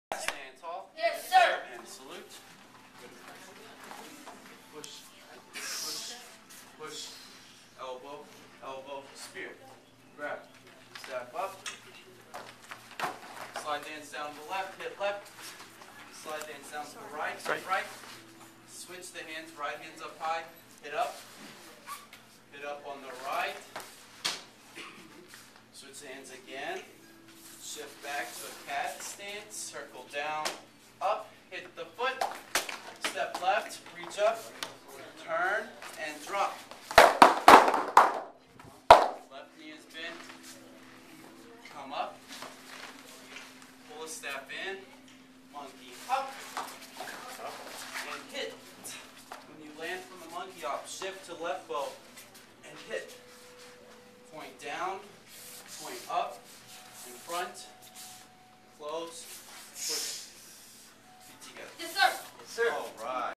Basic Kung Fu - Striking Staff Set #2
Mr Kassens leads the Basic Striking Staff set with instructions.